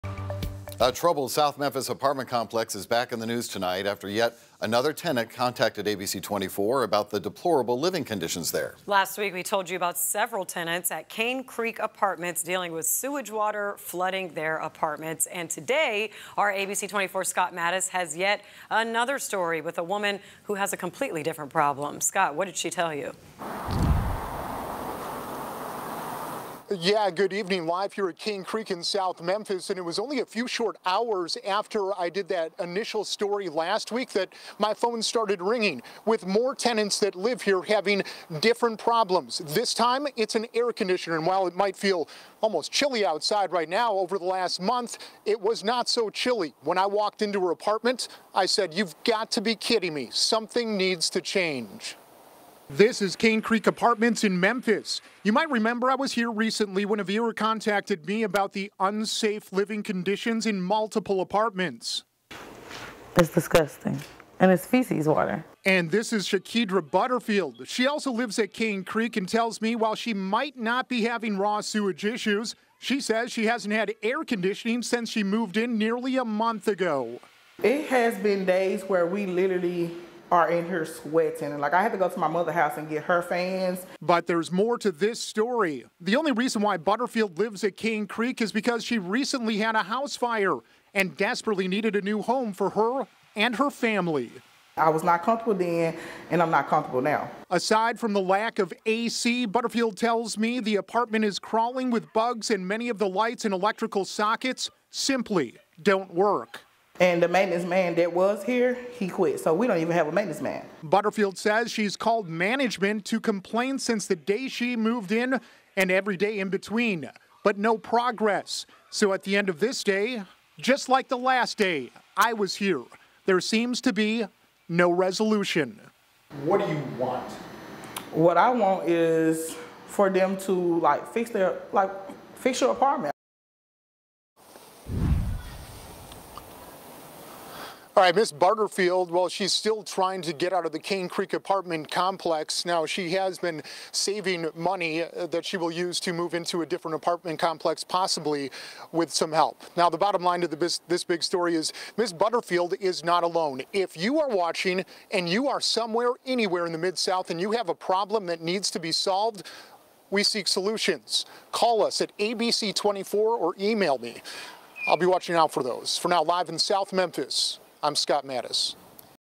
Memphis woman has not had air conditioning in almost a month | Cane Creek Apartments
A Cane Creek Apartments tenant is fed up with management not fixing her apartment, which is crawling with bugs and has no AC.